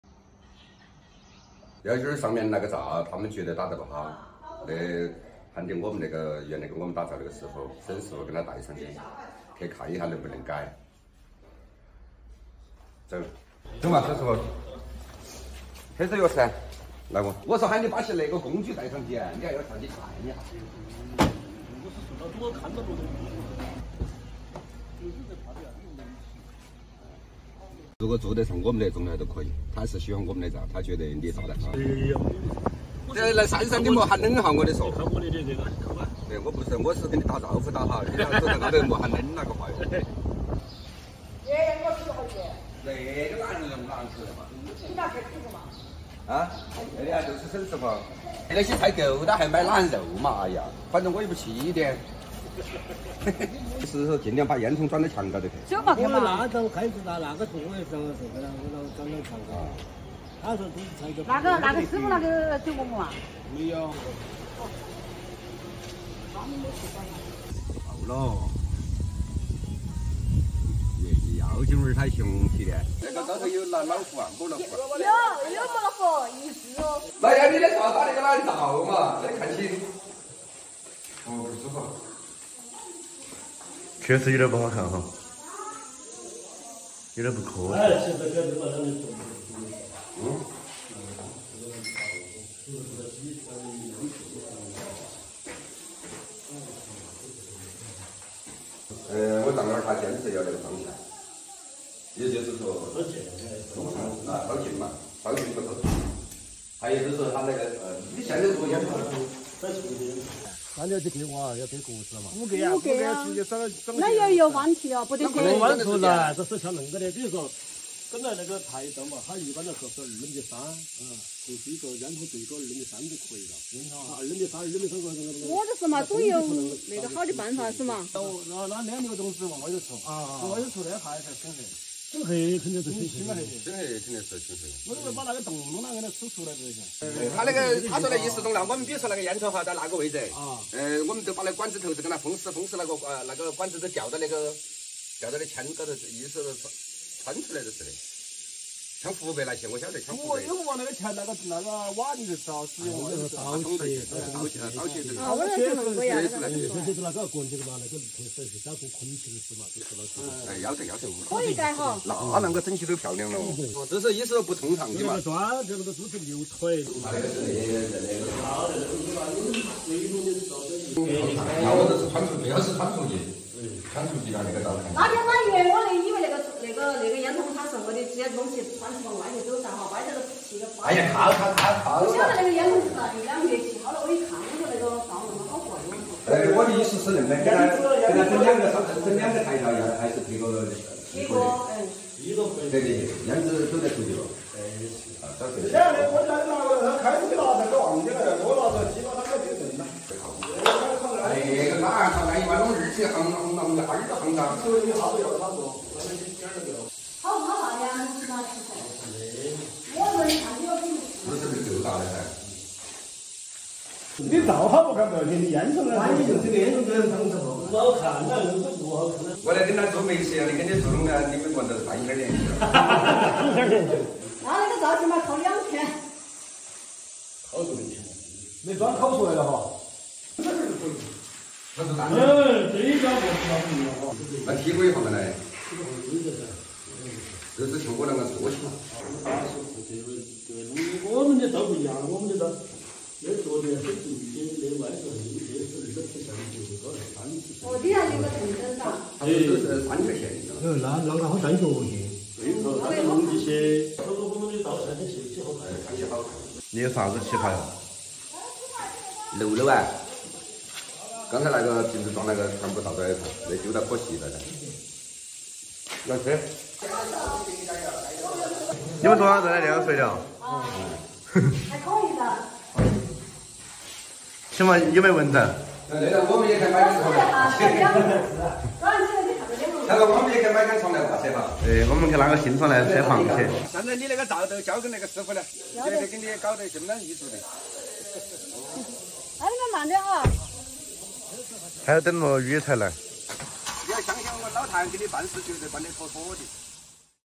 舅媽家的新灶嫌打得不夠好，老譚帶師傅去查看，準備拆了重新做！ 【小小譚】
么舅上面那個灶他們覺得打得不好，現在喊原來跟我們打灶這個師傅，去看一看能不能改

【小小譚】
小小譚 生活日常 vlog 我们强调了合法害虫控制方法的重要性，视频内容绝对无动物受害。